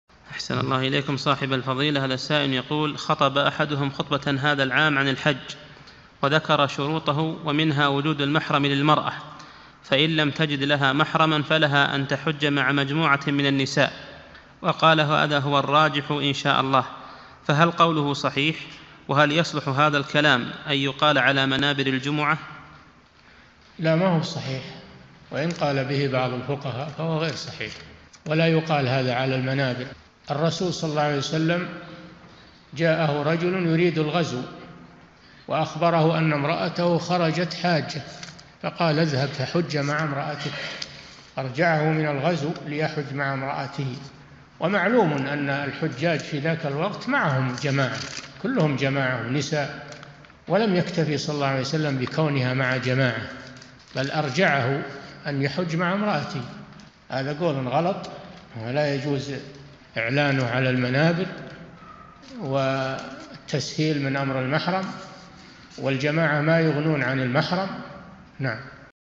[688 -954] قال خطيب الجمعة: إذا لم تجد المرأة لها محرما في الحج، فلها أن تحج مع مجموعة من النساء..!
-[688 /954] السؤال: خطب أحدهم خطبةً هذا العام عن الحج، وذكر شروطه ومنها: وجود المحرَم للمرأة، فإن لم تجِد لها محرمًا، فلها أن تحج مع مجموعةٍ من النساء، وقال: هذا هو الراجح -إن شاء الله- فهل قوله صحيح، وهل يصلح هذ الكلام أن يُقال على منابر الجمعة؟
الجواب: لا ما هو بصحيح، وإن قال به بعض الفقهاء فهو غير صحيح، ولا يُقال هذا على المنابر، الرسول ﷺ جاءه رجلٌ يُريد الغزو، وأخبره أن امرأته خرجت حاجة، فقال: «اذْهَب فحُج مع امرأتك» أرجعه من الغزو ليحج مع امرأته، ومعلوم أن الحجاج في ذلك الوقت معهم جماعة، كلهم جماعة ونساء، ولم يكتف ﷺ بكونها مع جماعة، بل أرجعه أن يحج مع امرأته، هذا قولٌ غلط، ولا يجوز إعلانه على المنابر، والتسهيل من أمر المحرَم، والجماعة ما يغنون عن المَحْرَم.
للاشتراك في قناة الشيخ صالح الفوزان
سؤال وجواب من شرح منظومة الآداب ( 945 ) مقطع

للاستفسار والإقتراحات والدعم
رئيس الفريق
لمشروع كبار العلماء